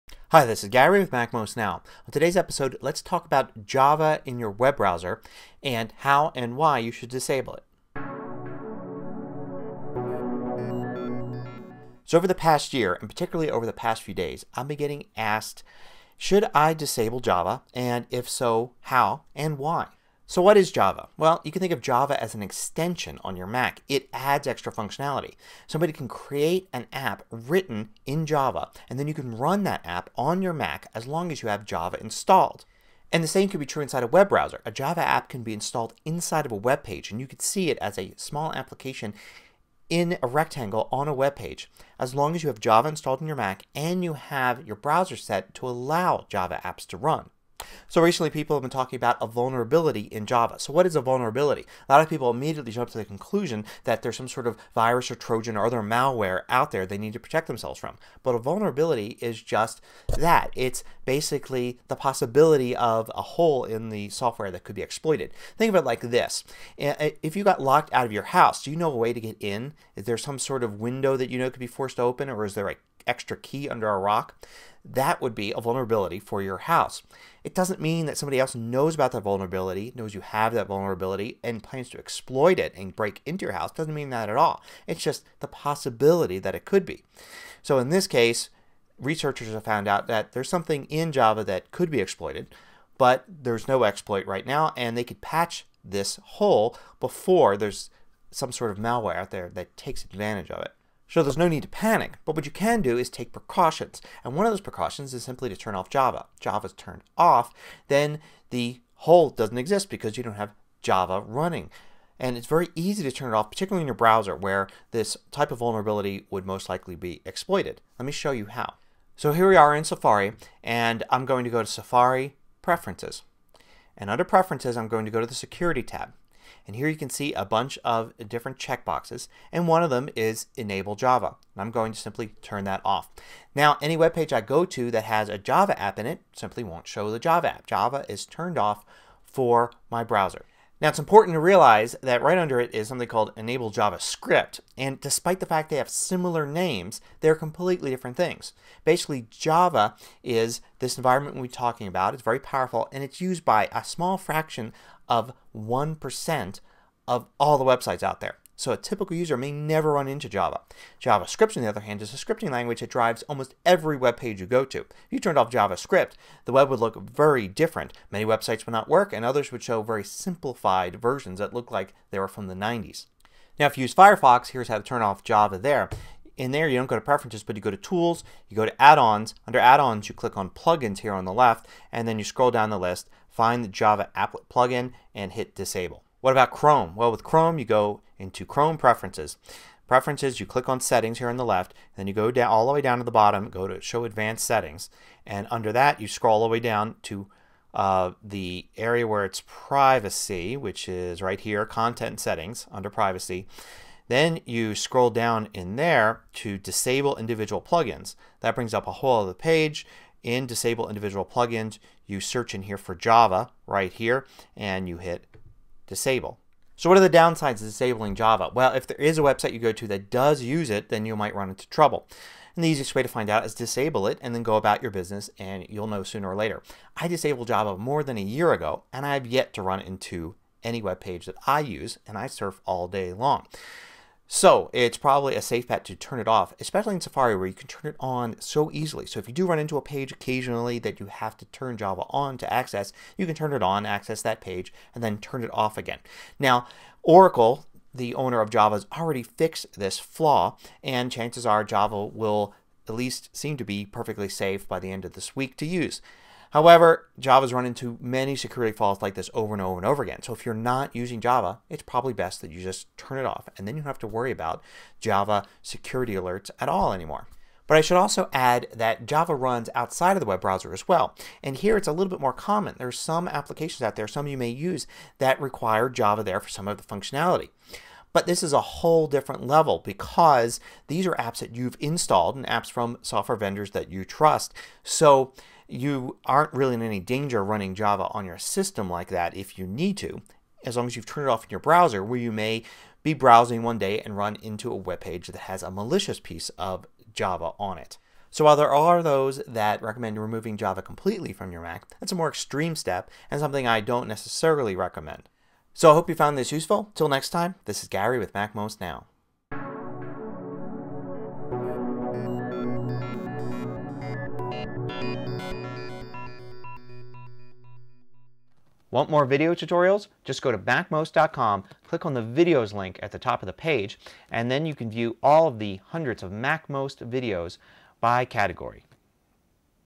Disable Java In Your Browser (MacMost Now 813)
It is easy to disable Java in your web browser. Doing so may help protect you from future malware threats. Java is no longer used on most web sites and you will most likely not notice the difference once you turn Java off.